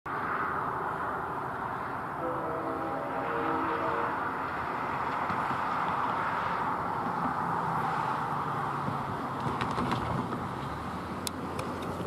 Distant Audio of CSX 6504's RS5T 4/17/19
Just a quick clip of CSX 6504's RS5T being blown in Rockdale, IL.  Sorry I couldn't get more audio of it.